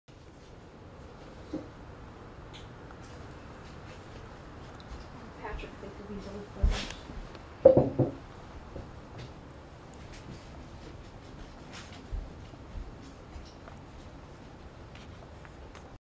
Dr Marks Brauhaus 002 2
15 gallons of Asian pear cider. The bucket has white wine yeast, the 2 carboys have sparkling wine yeast. Temperature in the storage area is 60'